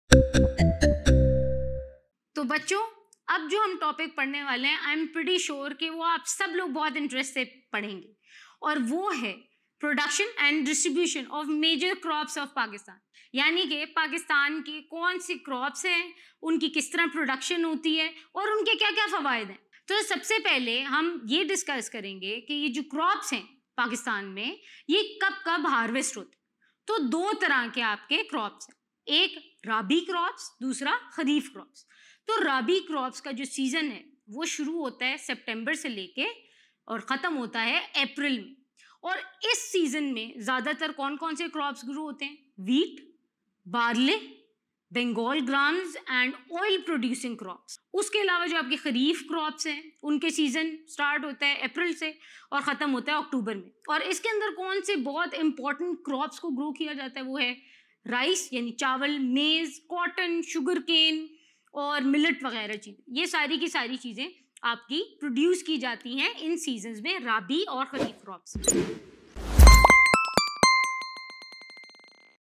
Pak Studies Class 10 | Chapter 3 | Topic 25 | Production and Distribution of Major Crops of Pakistan
Learn the Economic Development of Pakistan of class 10 here!
Do you want to learn about Pakistan’s economic development phases?

This chapter covers the major aspects such as Pattern of Modernization in Agriculture, The Use of Good Quality Seeds, Pesticides /insect killer, Improvement in the System of Irrigation, Water Logging and Salinity, New Roads, Main Problems Associated with Agriculture ( Lack of Agricultural Inputs, Inefficient use of Cultivable Area In Pakistan, Natural Disasters, Inadequate Irrigation Facilities, Lack of Modernized Cultivation In Pakistan, Lack of Agricultural Credit /Praise, Water Logging and Salinity, Lack of Efficient Means of Transportation, Plant Diseases, Backwardness of Farmers, and Prices of Agricultural Goods. The chapter also includes basic Solutions of Agricultural Problems, Industries of Pakistan like Cottage Industries, Small Industries, and Large Scale Industries.

In this video, you will study Pak Studies Grade 10 Chapter 3 Topic 25 "Production and Distribution of Major Crops of Pakistan" by instructor Beenesh Ansari.